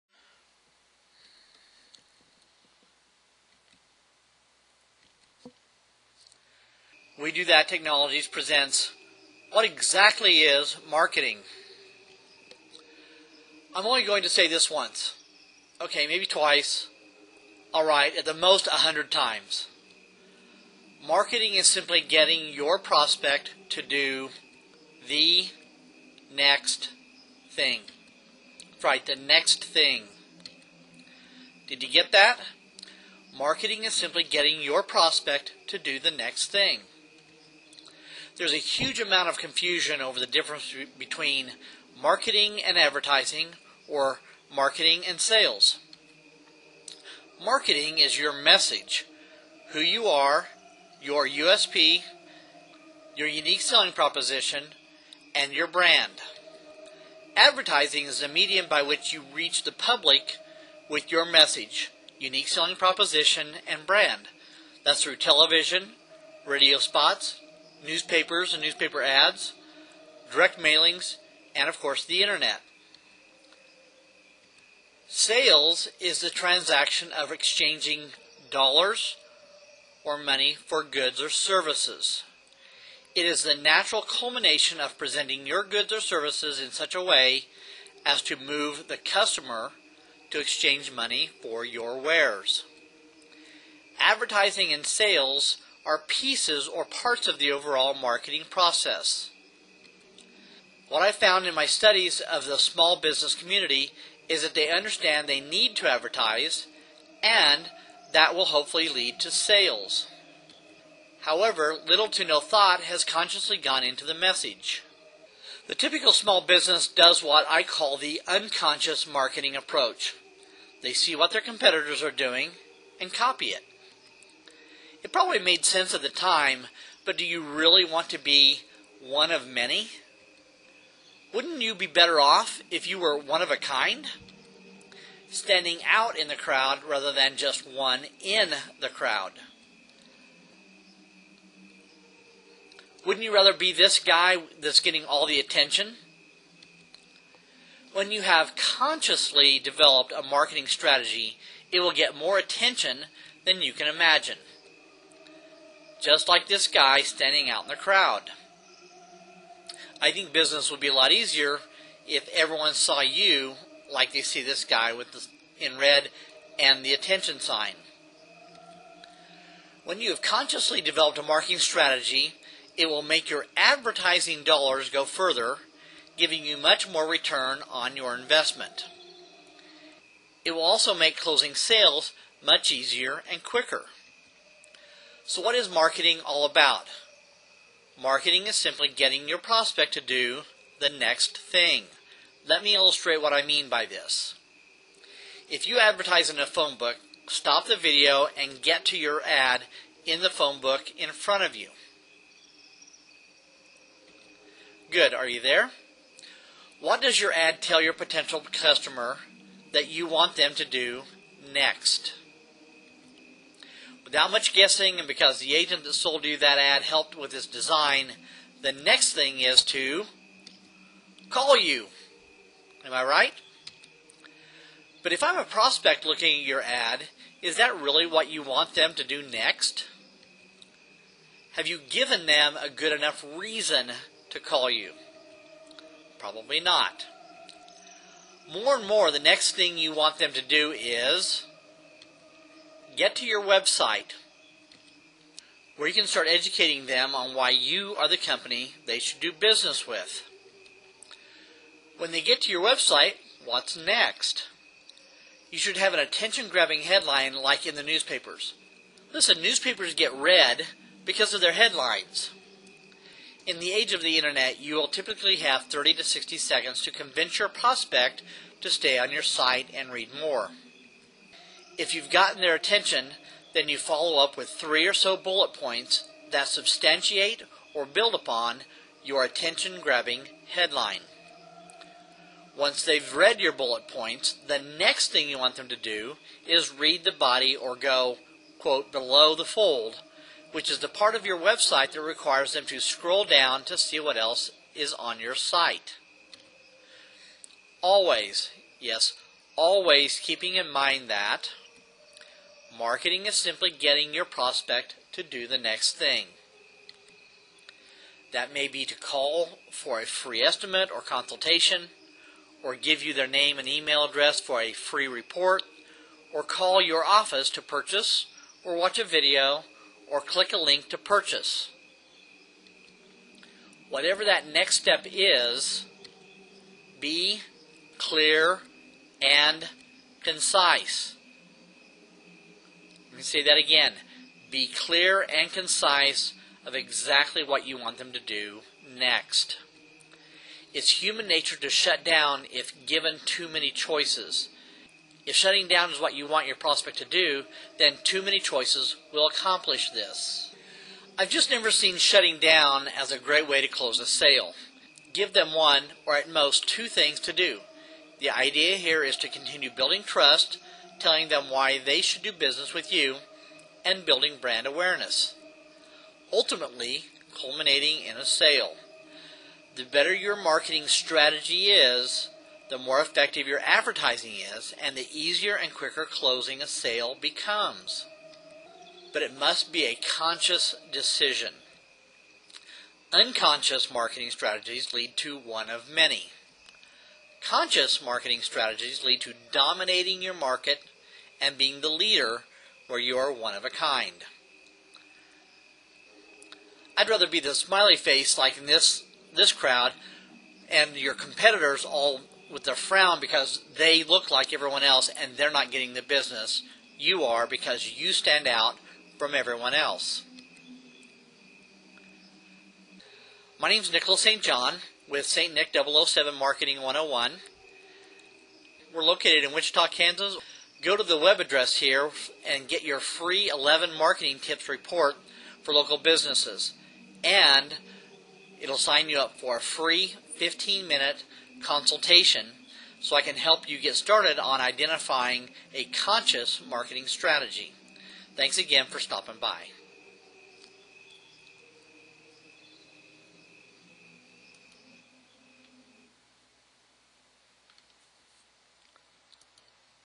stnick007 - What is Marketing?
If you are not sending a clear message of who you are, how you are different from all your competitors, and walking your Prospect step by step through the entire process.  So if your ads or website give people SEVERAL things to do NEXT, you may just find yourself violating the primary objective of Marketing.
The Next Thing!